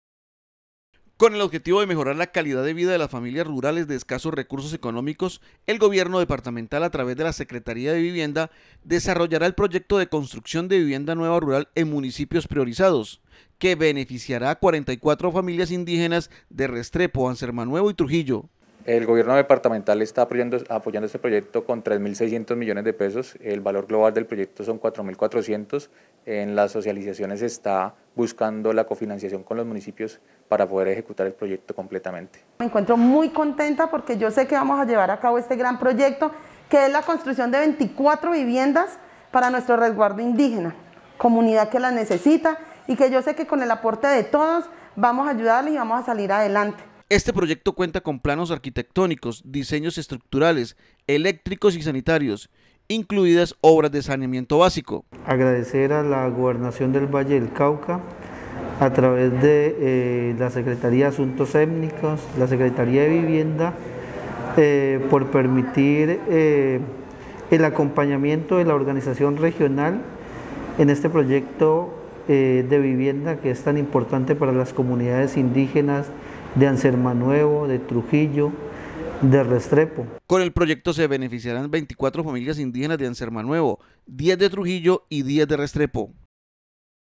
Proyecto de la Gobernación del Valle dará a 44 familias indígenas vivienda digna.
Emitido el  27 de  febrero 2021 Dia por el Canal regional Telepacífico.